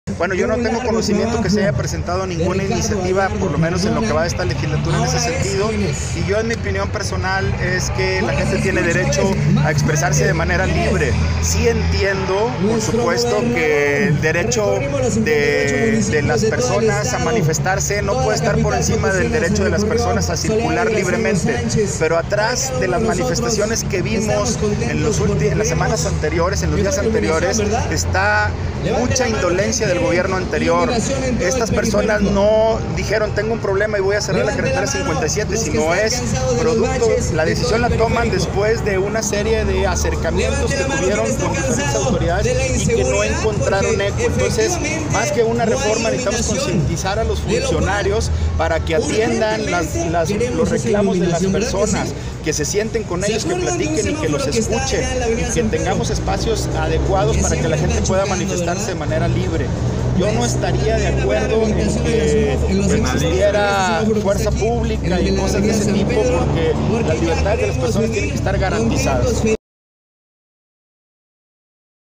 Dip José Luis Fernández Martínez
Se debe privilegiar el diálogo en manifestaciones.